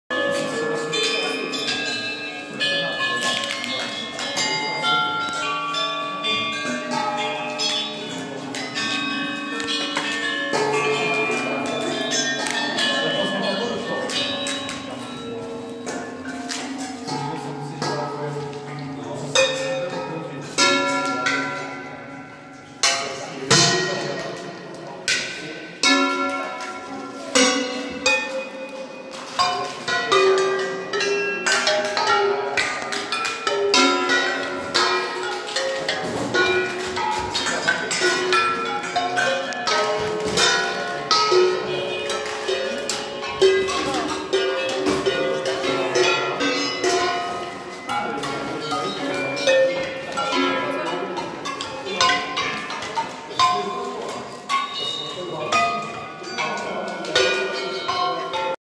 Výtvarně-hudební dílna Zahrada Snivců - Jiří Janda a Jaroslav Kořán (ukázka)
ze souboru V ZAHRADĚ - Perkusní interaktivní malba,
Hudebně - výtvarná dílna "Zahrada snivců"
Jiří Janda & Jaroslav Kořán, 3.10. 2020, Galerie Špejchar, Chomutov
Foto: J. Zdráhalová
Kamera, komentář, střih: JJanda